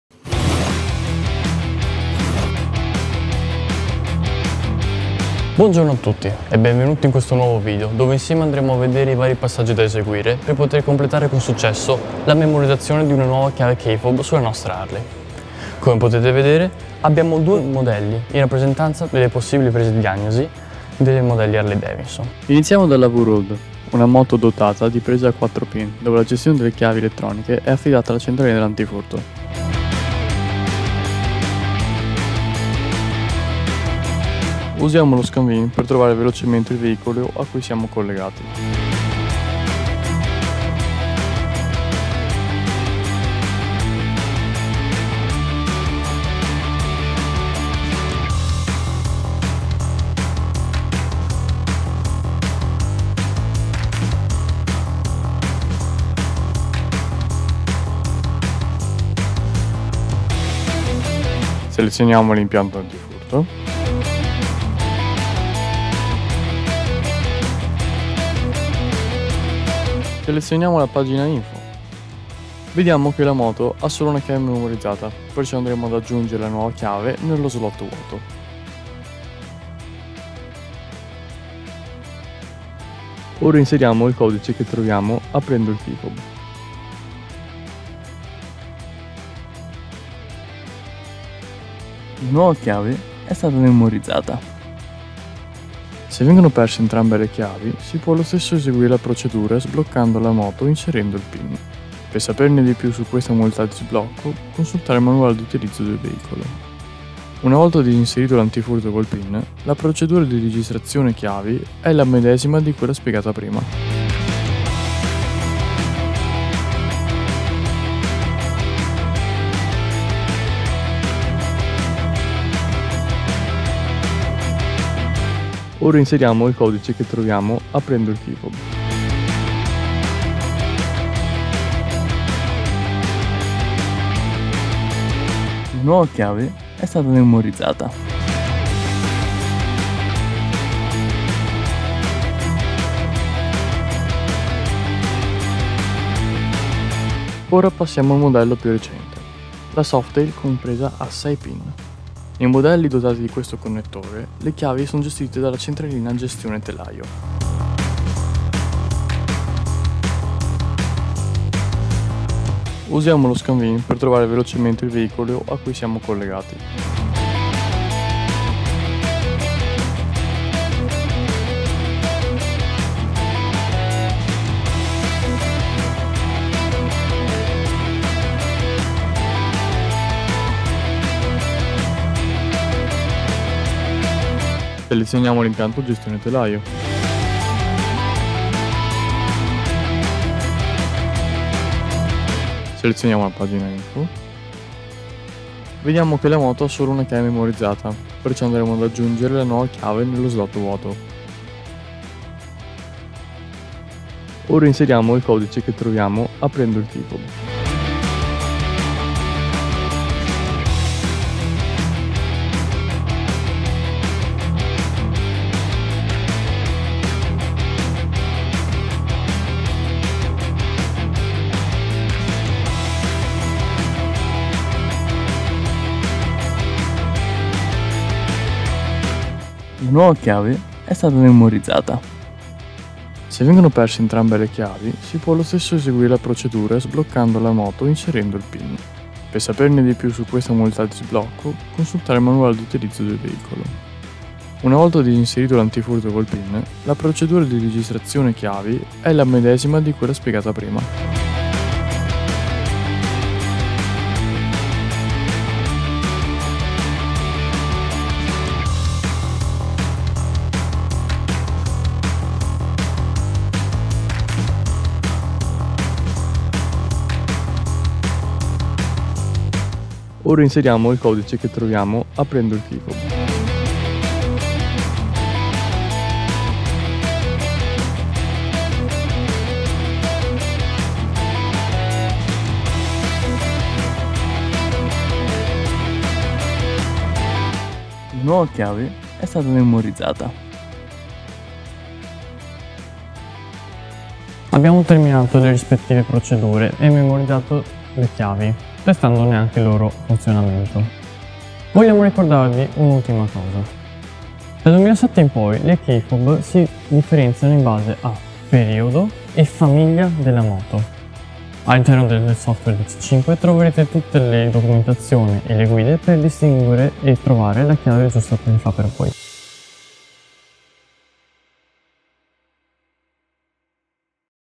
IT - Riprogrammazione KeyFob Harley Davidson
TEXAbike - Torna lo Specialista diagnosi BIKE con un nuovo video-tutorial, utile a fare chiarezza sull'argomento "riprogrammazione KeyFob @Harley Davidson".

Nel corso del video vengono descritte le procedure da eseguire per portare a termine tale operazione. Le protagoniste sono una VRod (con presa a 4 pin) e una più recente Softail (con presa a 6 pin), in rappresentanza delle possibili prese diagnosi Harley Davidson. Buona visione!